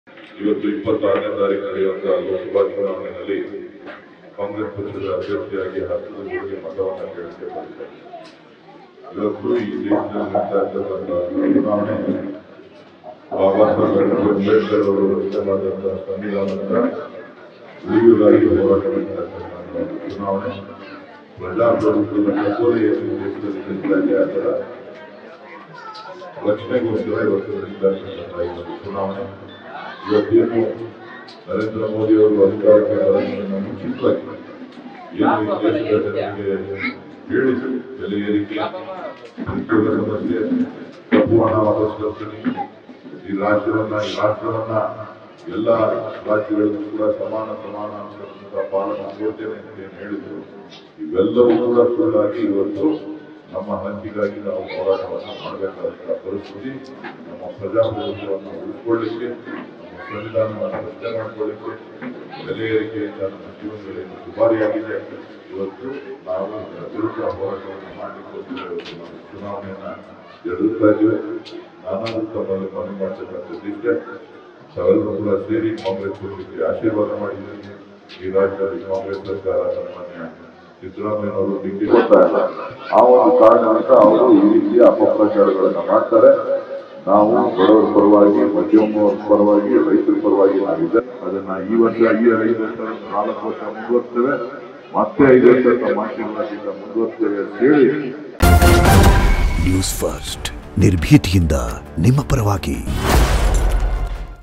Congress​ನಿಂದ ಕೊಟ್ಟ ಎಲ್ಲಾ ಭರವಸೆ ಈಡೇರಿಸ್ತೀವಿ..| DK Suresh | Lok Sabha Election 2024 | Newsfirst
Congress​ನಿಂದ ಕೊಟ್ಟ ಎಲ್ಲಾ ಭರವಸೆ ಈಡೇರಿಸ್ತೀವಿ..| DK Suresh | Lok Sabha Election 2024 | @newsfirstkannada